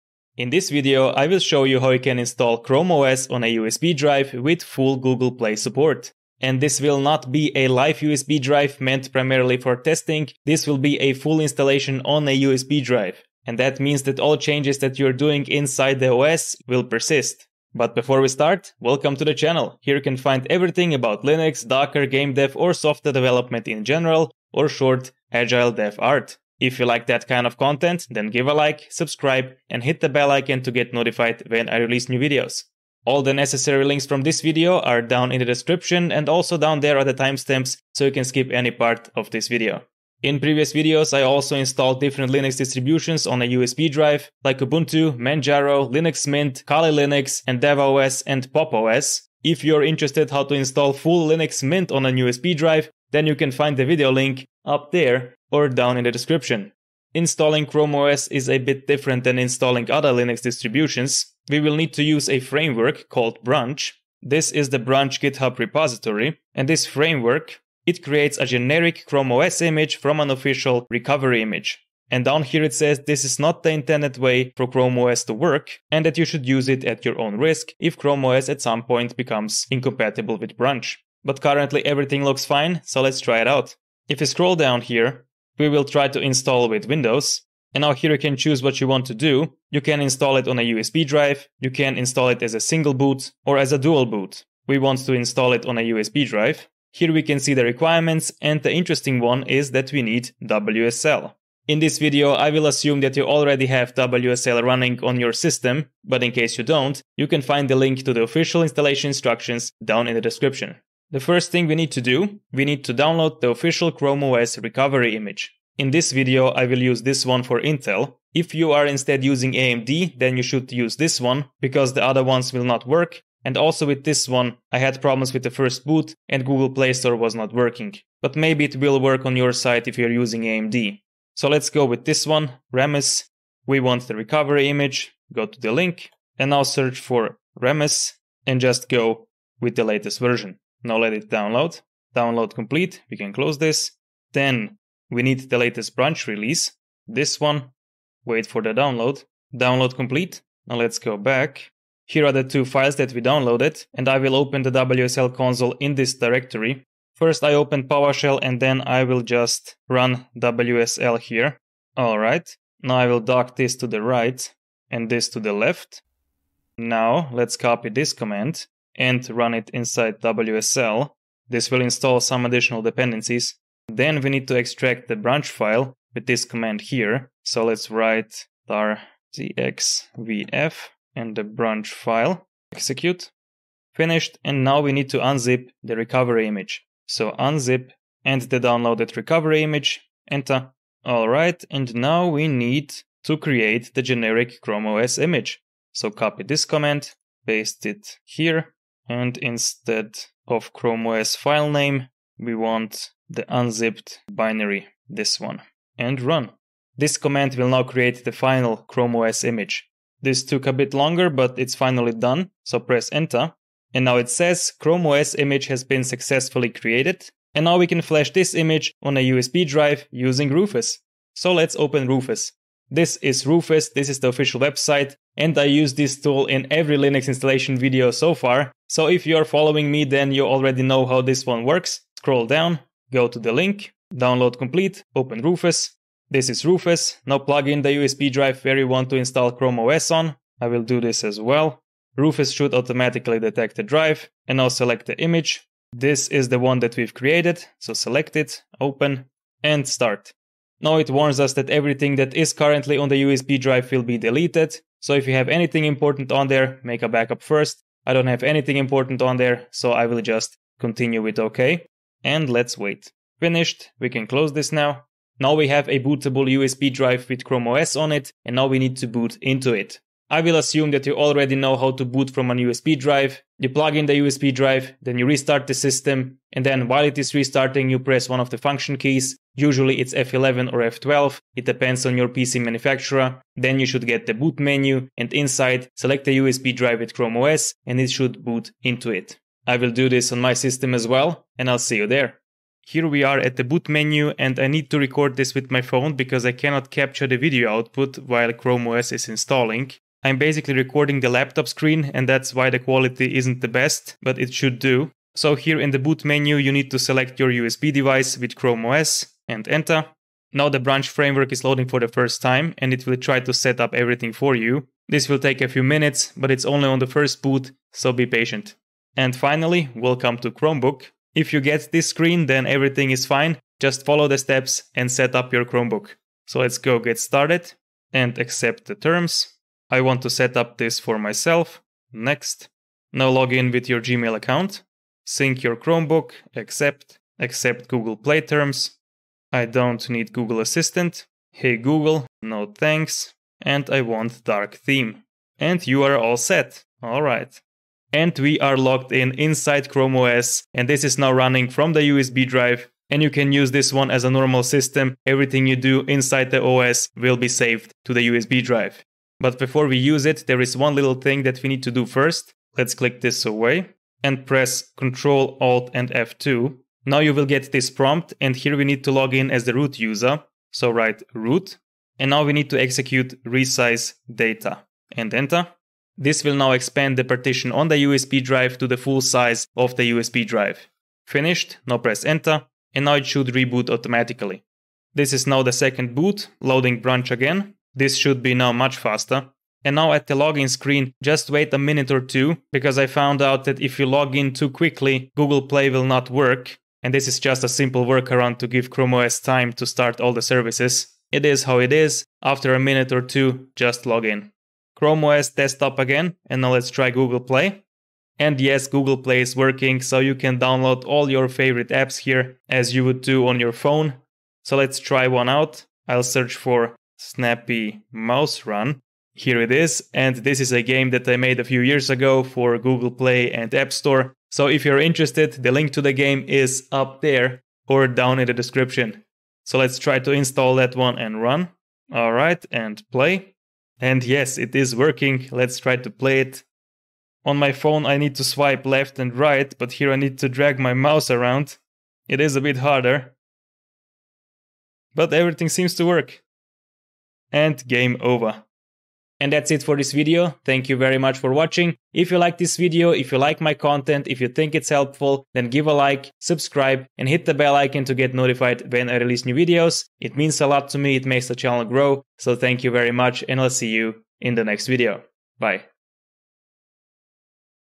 Install Full ChromeOS with Google Play support on a USB Drive (2022)
Install Full Persistent ChromeOS Desktop with Google Play support on a USB drive.

00:00 Introduction
01:04 Prepare Brunch Framework ISO
03:43 Create ChromeOS USB with rufus
04:52 ChromeOS USB first boot setup
06:4 Run ChromeOS  Google Play
06:59 Run Google Play App in ChromeOS